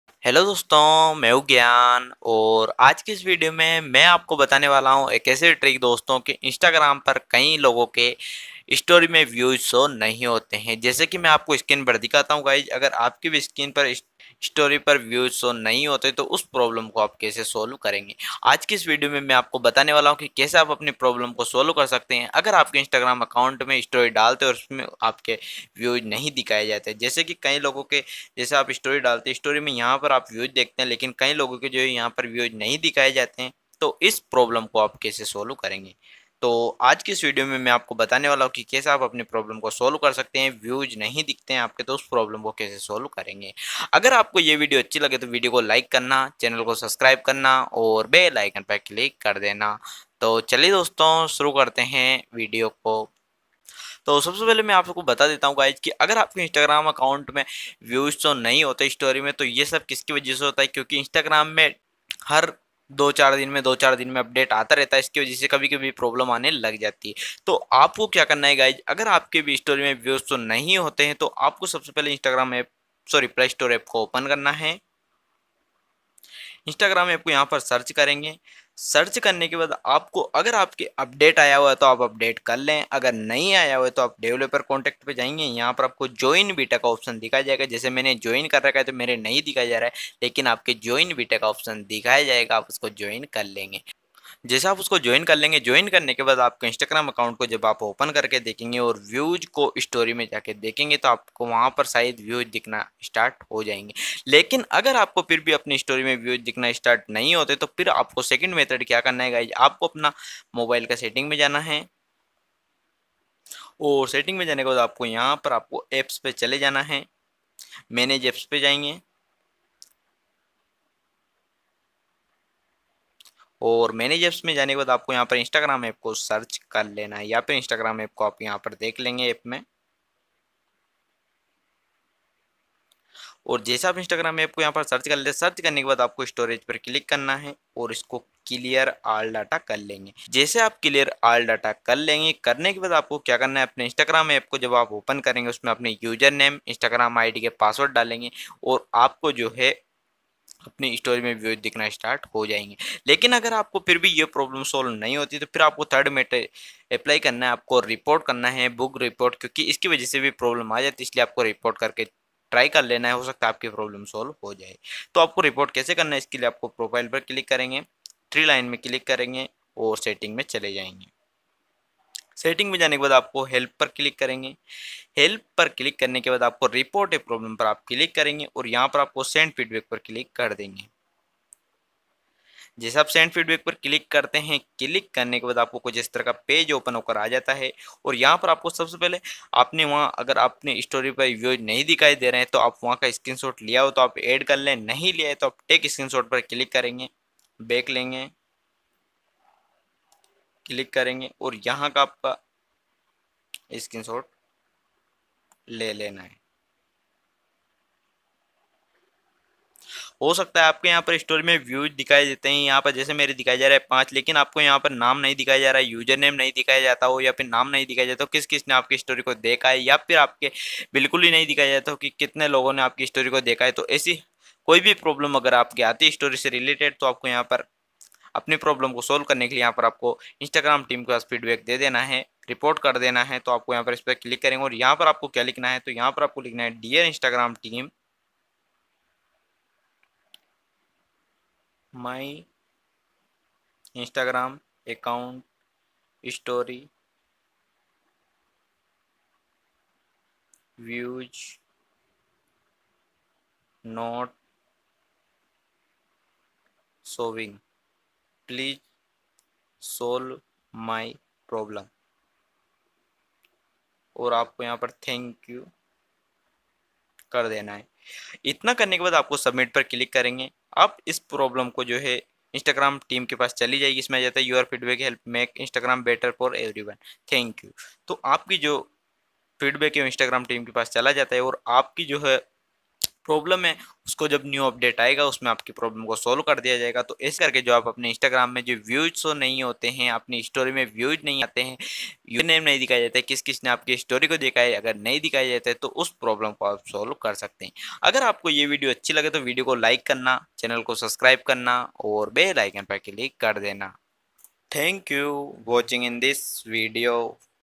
instagram story views not showing 2022 || instagram story id Views Name not showing
:--Aaj ki video me aapko me batane vala hu ki Instagram Story Views Not Showing:-